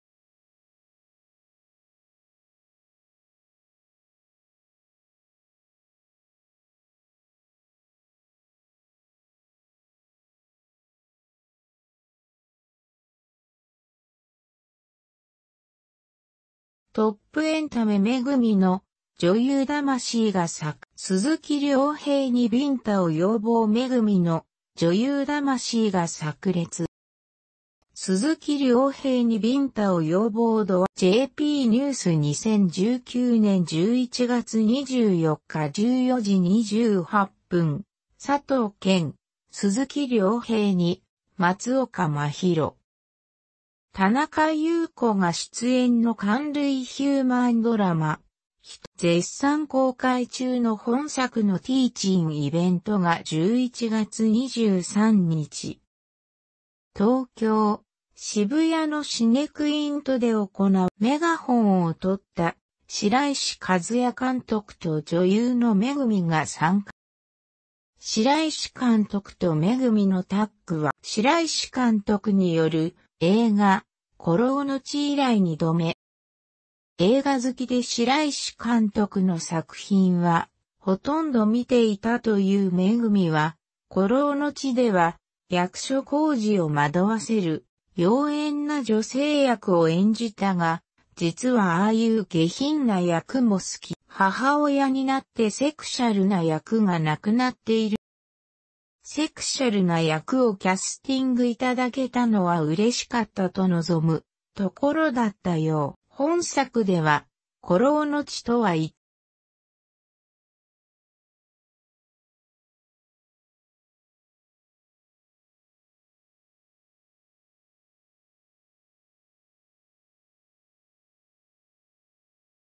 MEGUMIの女優魂が炸裂、鈴木亮平にビンタを要望 - 趣味女子を応援するメディア「めるも」
TOP エンタメ MEGUMIの女優魂が炸裂、鈴木亮平にビンタを要望 MEGUMIの女優魂が炸裂、鈴木亮平にビンタを要望 dwango.jp news 2019/11/24 14:28 佐藤健、鈴木亮平、松岡茉優、田中裕子が出演の感涙ヒューマンドラマ『ひとよ』。絶賛公開中の本作の